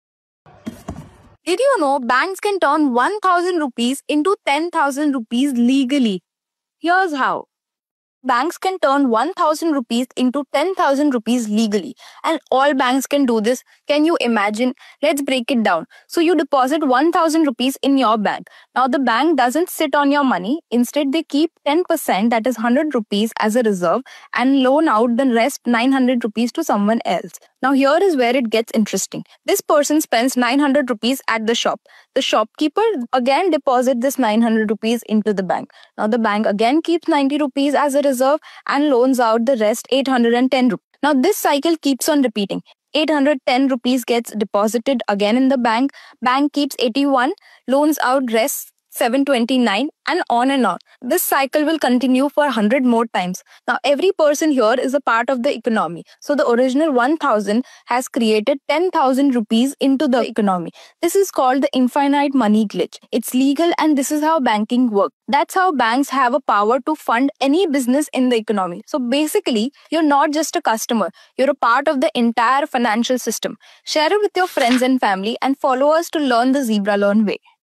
Did you know banks can turn ₹1,000 into ₹10,000 legally?
This happens through fractional reserve banking, where banks keep 10% of deposits as reserves while lending the rest. These reserves serve three crucial purposes:

Meeting daily customer withdrawals
Following central bank requirements
Maintaining emergency funds for unexpected events

Here's how it works: You deposit ₹1,000. The bank keeps ₹100 (10%) as reserve and lends ₹900. That ₹900 gets spent and redeposited at another bank, which keeps ₹90 and lends ₹810. This cycle continues, eventually turning your ₹1,000 into ₹10,000 in the economy - all while keeping enough reserves to ensure the banking system stays stable and secure.